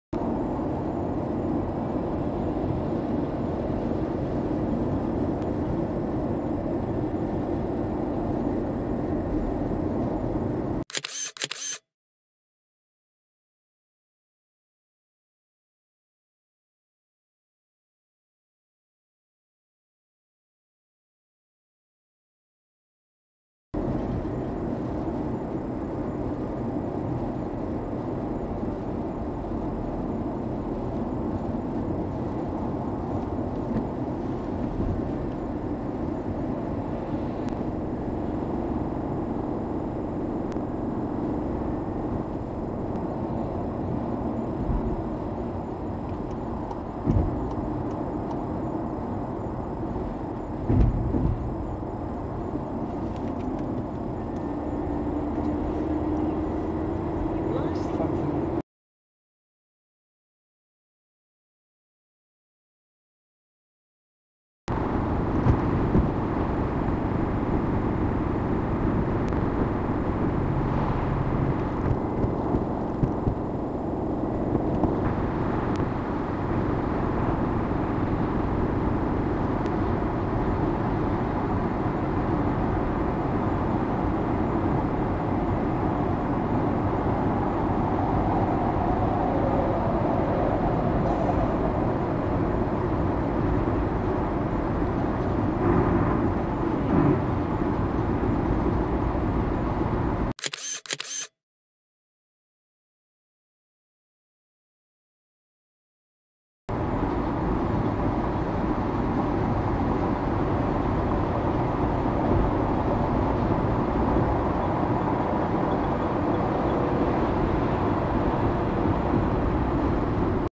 Catching up with a tanker carrying aeroplane fuel. M1 motorway, City of Gold Coast.
Catching up with a tailgating tanker, carrying aeroplane gasoline, along the M1. What was the point of tailgating so dangerously?

The idiot SUV driver in front was at fault for not keeping right ... but what was the gain ... imagine if the idiot SUV driver did a "brake check" ... boom!  B I G    B O O M !!

Pacific Motorway (M1), City of Gold Coast, Queensland, Australia